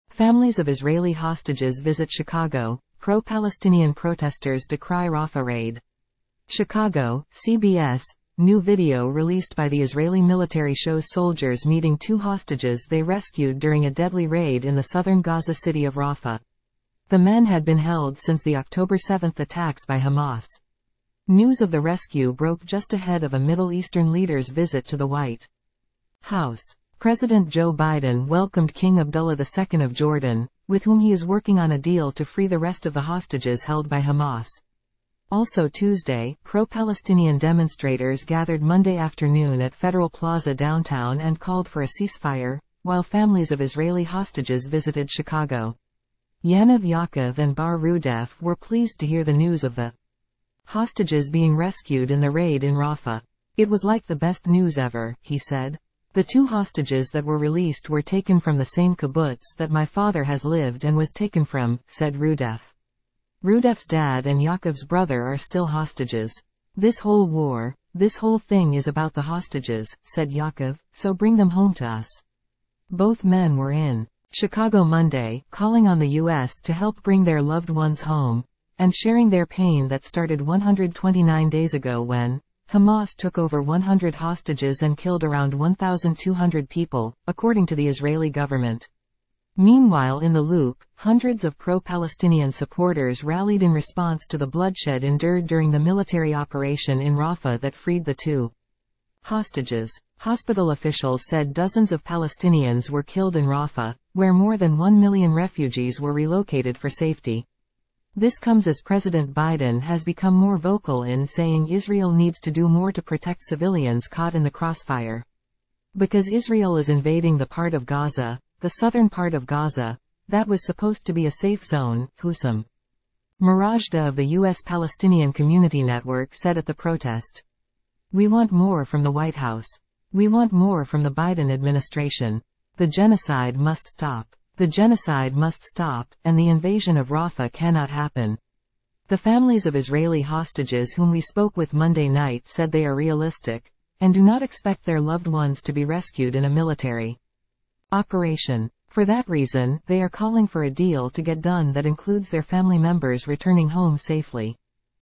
Families of Israeli hostages visit Chicago pro-Palestinian protesters decry Rafa raid
CHICAGO (CBS) -- New video released by the Israeli military shows soldiers meeting two hostages they rescued during a deadly raid in the southern Gaza city of Rafa.

The men had been held since the Oct. 7 attacks by Hamas.

News of the rescue broke just ahead of a Middle Eastern leader's visit to the White House. President Joe Biden welcomed King Abdullah II of Jordan - with whom he is working on a deal to free the rest of the hostages held by Hamas.

Also Tuesday, pro-Palestinian demonstrators gathered Monday afternoon at Federal Plaza downtown and called for a cease-fire - while families of Israeli hostages visited Chicago.

Yaniv Yaakov and Bar Rudaeff were pleased to hear the news of the hostages being rescued in the raid in Rafa.

"It was like the best news ever," he said.

"The two hostages that were released were taken from the same kibbutz that my father has lived and was taken from," said Rudaeff.

Rudaeff's dad and Yaakov's brother are still hostages.

"This whole war - this whole thing- is about the hostages," said Yaakov, "so bring them home to us."

Both men were in Chicago Monday, calling on the U.S. to help bring their loved ones home - and sharing their pain that started 129 days ago when, Hamas took over 100 hostages and killed around 1,200 people, according to the Israeli government.

Meanwhile in the Loop, hundreds of pro-Palestinian supporters rallied in response to the bloodshed endured during the military operation in Rafa that freed the two hostages.

Hospital officials said dozens of Palestinians were killed in Rafa, where more than 1 million refugees were relocated for safety.

This comes as President Biden has become more vocal in saying Israel needs to do more to protect civilians caught in the crossfire.

"Because Israel is invading the part of Gaza - the southern part of Gaza - that was supposed to be a safe zone," Husam Marajda of the U.S. Palestinian Community Network said at the protest. "We want more from the White House. We want more from the Biden administration. The genocide must stop. The genocide must stop, and the invasion of Rafa cannot happen."

The families of Israeli hostages whom we spoke with Monday night said they are realistic - and do not expect their loved ones to be rescued in a military operation. For that reason, they are calling for a deal to get done that includes their family members returning home safely.